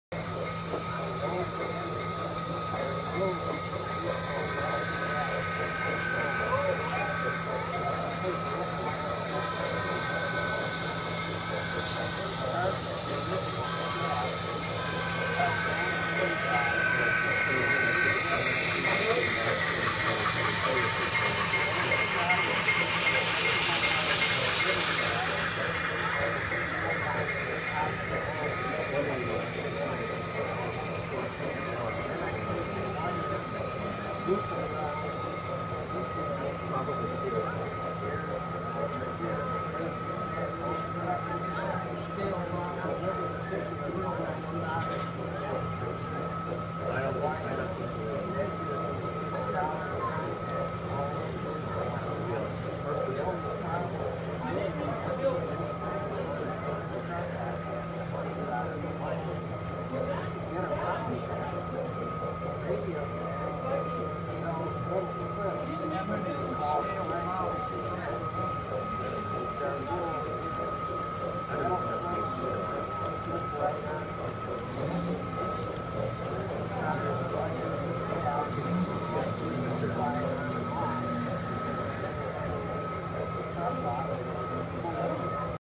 SCL Pulling For You Open House 11-29-2014
My new Seaboard Coast Line ALCO C430 and EMD SD45-2 pulls a freight at our Thanksgiving Open house on the Grand Strand Model Railroad Club.